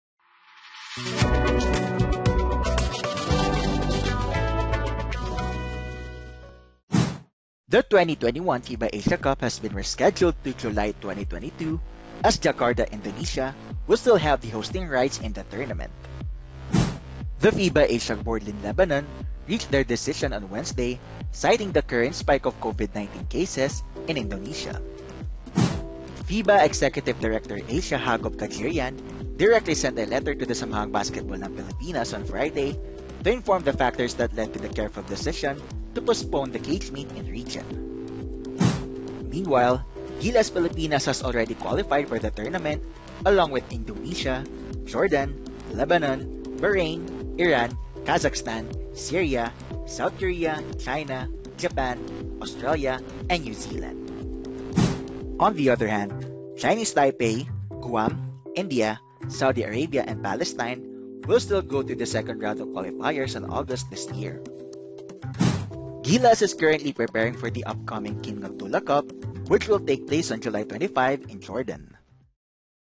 FIBA reschedules 2021 Asia Cup to July 2022 due to rising COVID-19 cases in Indonesia
The 2021 FIBA Asia Cup has been rescheduled to July 2022, as Jakarta, Indonesia will still have the hosting rights in the tournament.